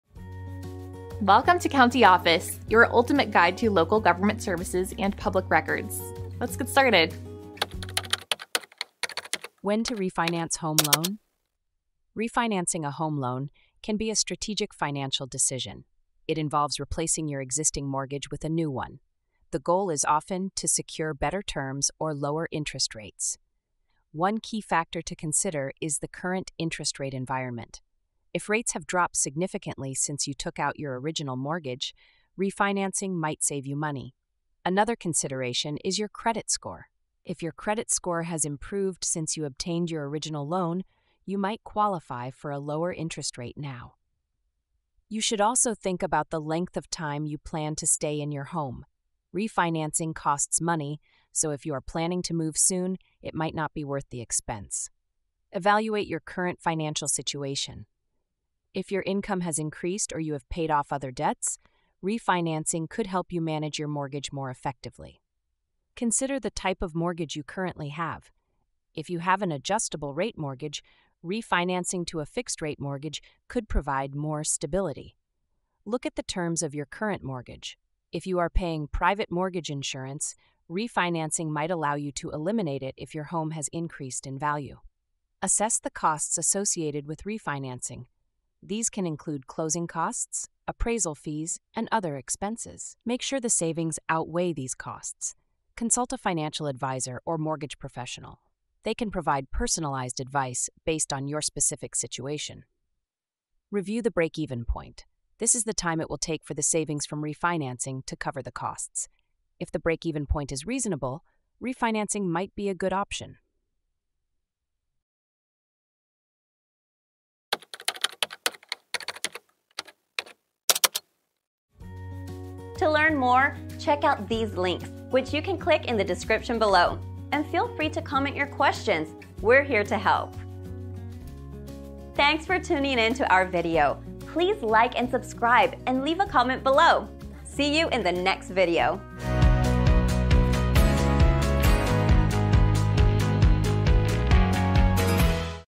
When to Refinance Home Loan? - CountyOffice.org
When to Refinance Home Loan? Have you ever considered refinancing your home loan but weren't sure when the right time is? In this insightful video, we delve into the key factors to consider when deciding whether to refinance your mortgage. From interest rate trends to your credit score, we break down the important considerations that can help you make an informed financial decision. Discover how refinancing could potentially save you money or provide more stability in your mortgage situation.

Consulting a financial advisor or mortgage professional can offer personalized guidance tailored to your specific circumstances. Understanding the costs associated with refinancing, such as closing fees and appraisal expenses, is crucial to ensuring that the benefits outweigh the expenses. We also explore the concept of a break-even point, the pivotal moment when your savings from refinancing surpass the costs incurred.

Join us for expert insights on navigating the refinancing process and making sound financial decisions for your home.